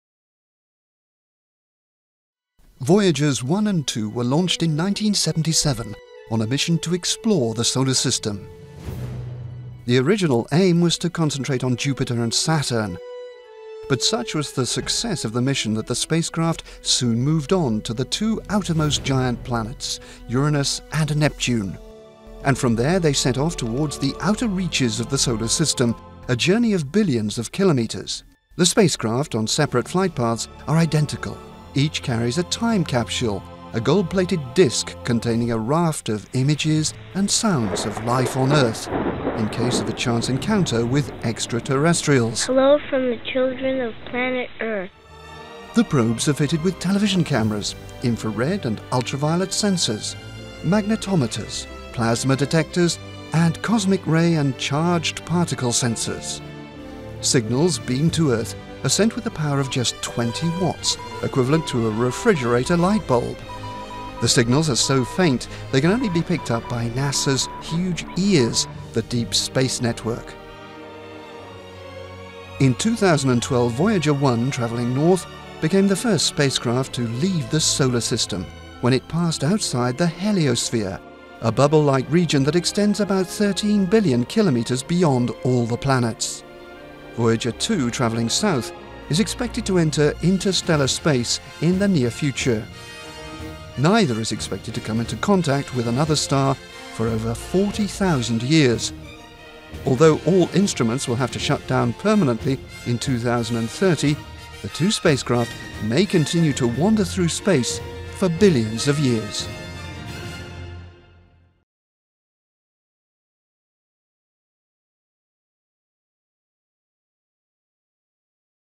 Voyager, the solar system and beyond
Videographic on the Voyager mission. NASA says Voyager 2 has passed into interstellar space.VIDEOGRAPHIC